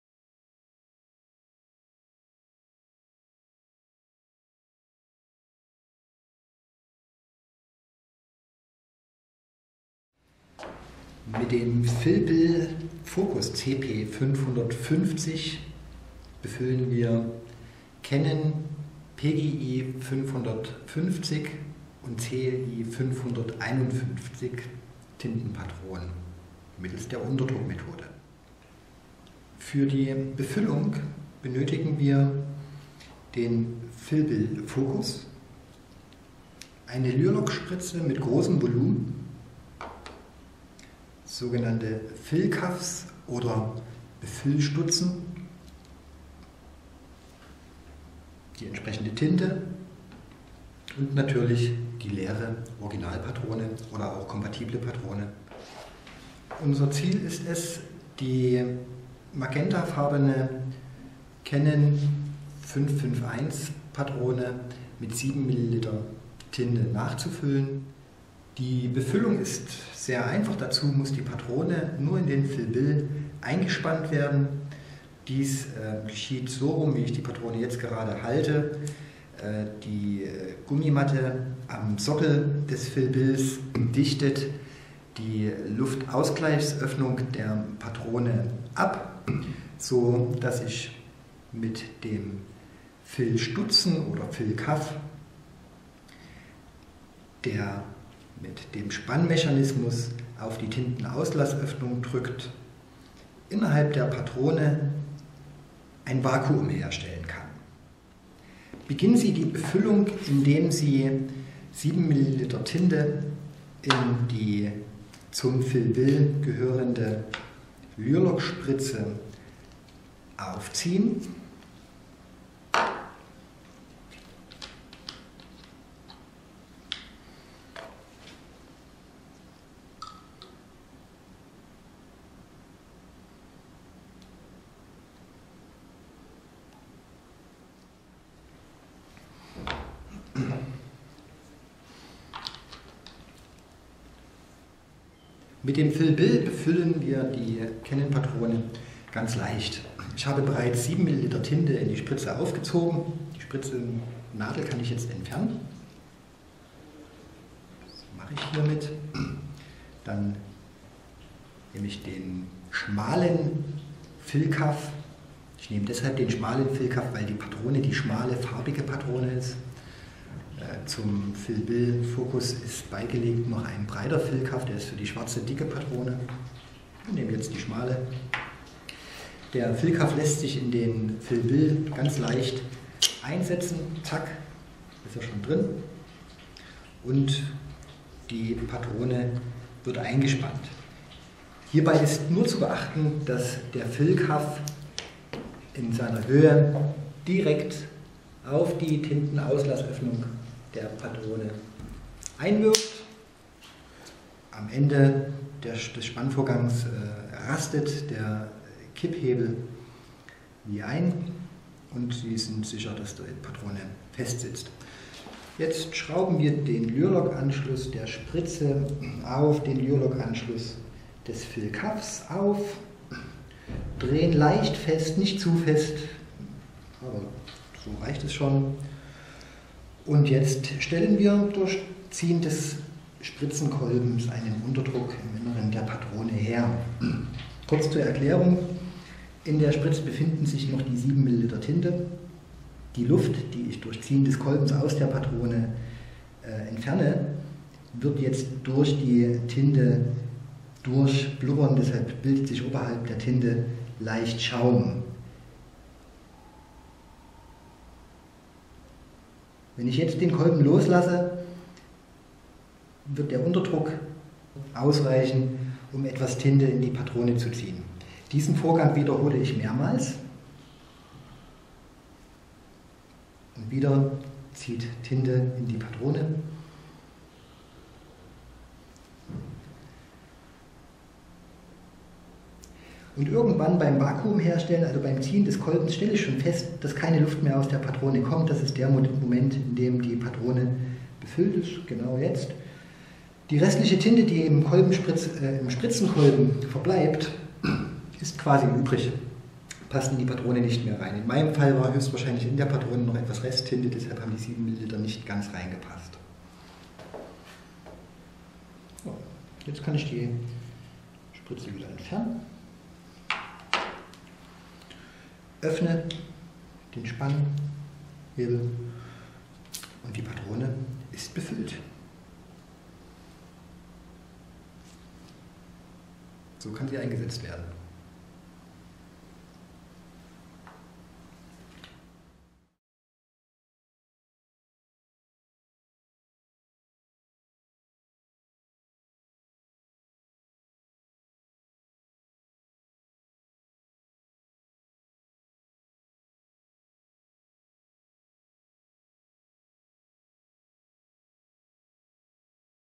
Canon PGI-550, CLI-551 nachfüllen mit Refill Tool
Canon PGI-550, CLI-551 Tintenpatronen ganz einfach mit Tinte befüllen. Wir zeigen Ihnen wie es simpel & clever mit dem Refill Tool funktioniert. Der Fill Bill FOCUS bei OCTOPUS